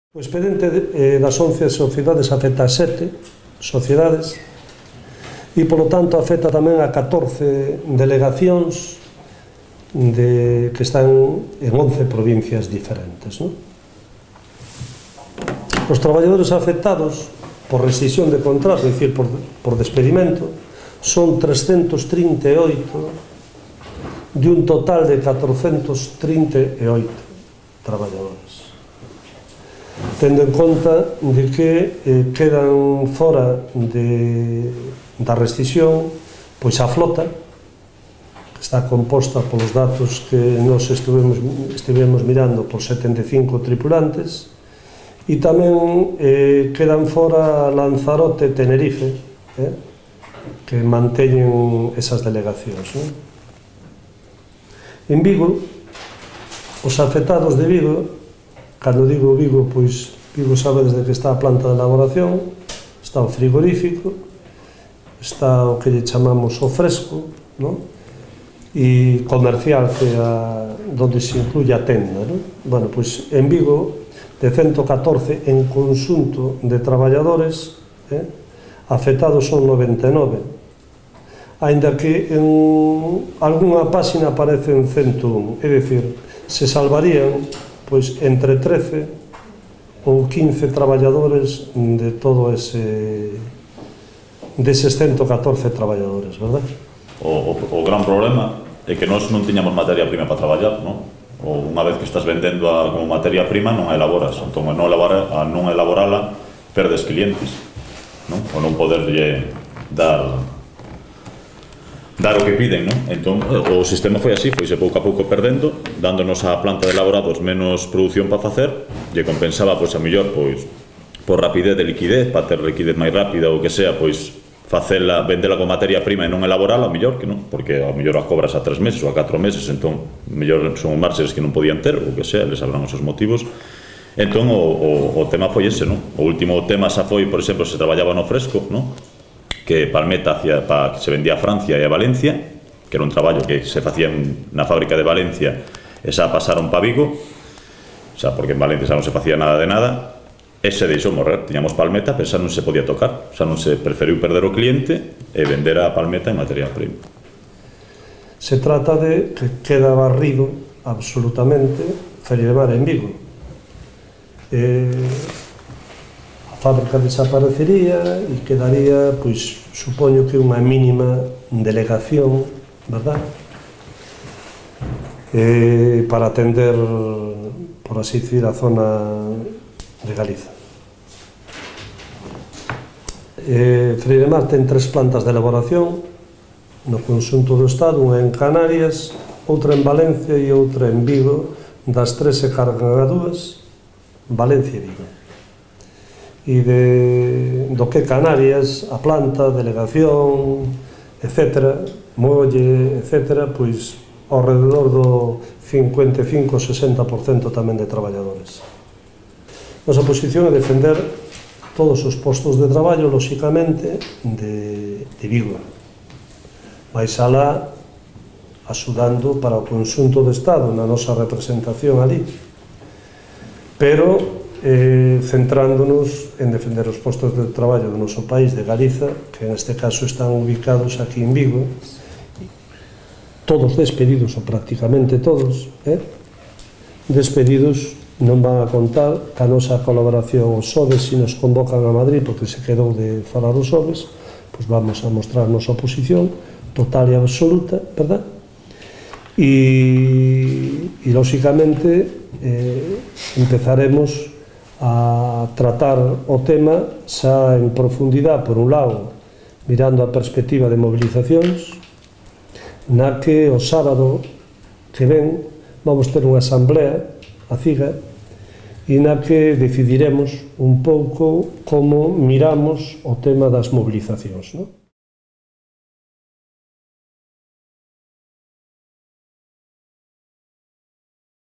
Freiremar, cuarta pesqueira estatal, quer despedir o 83% do persoal (rolda de prensa, 21/10)
A cuarta empresa pesqueira do Estado, Freiremar, pretender realizar un ERE ao 83% do seu cadro de persoal. A FGAMT-CIG prepara xa un calendario de mobilizacións, segundo indicou en conferencia de prensa o secretario nacional da federación, Xosé Fernández Piñeiro, que compareceu en compaña de membros do comité de empresa.